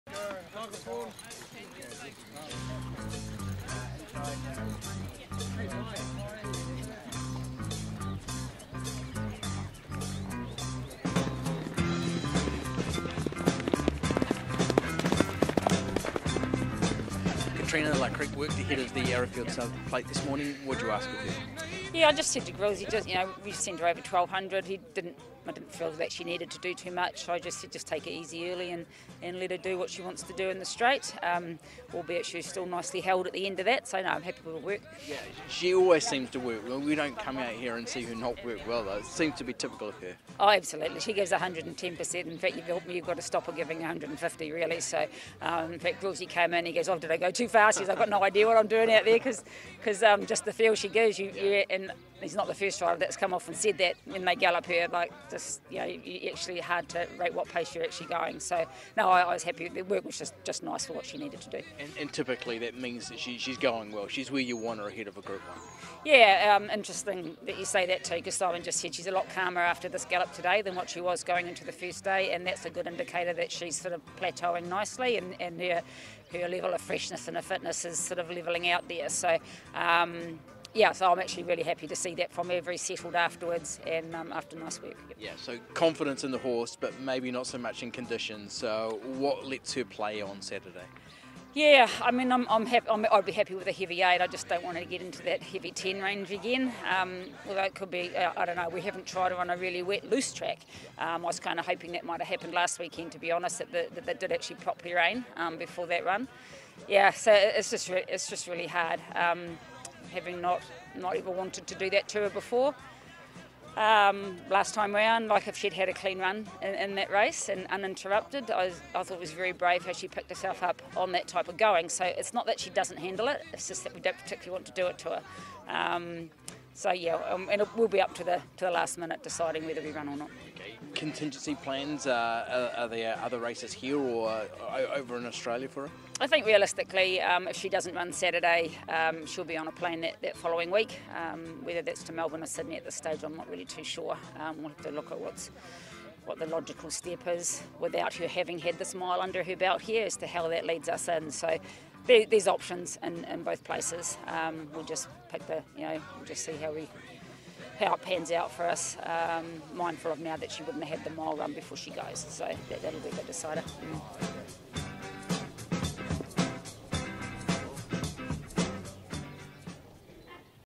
ARROWFIELD - LA CRIQUE
She's flying but will the weather play ball? Katrina Alexander with the latest on Arrowfield Stud Plate favourite La Crique